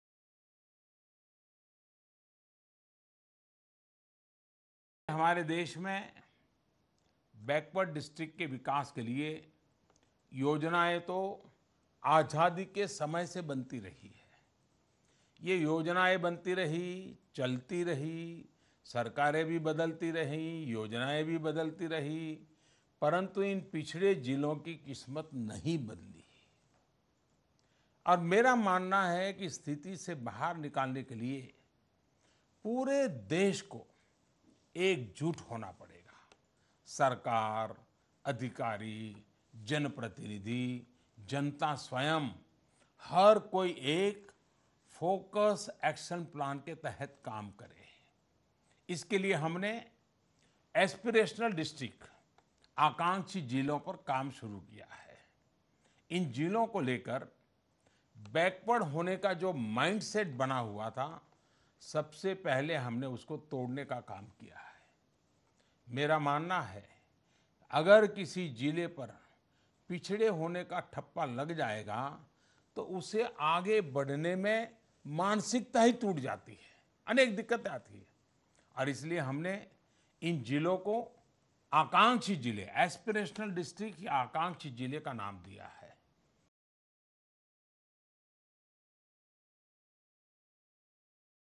PM Modi urges nation to work under a ‘Focus Action Plan’ for developing backward districts
New Delhi, Oct 17 (ANI): Prime Minister Narendra Modi on Wednesday urged that the nation should come together and work under a ‘Focus Action Plan’ in a move to develop backward districts. Speaking to the Bharatiya Janata Party (BJP) workers, Modi said, “The fate of backward districts has not changed even after number of governments changed along with their schemes for these districts. I believe that to take these districts out of this situation the whole nation should come together and work under a ‘Focus Action Plan’. The government has been calling backward districts as ‘aspirational districts’.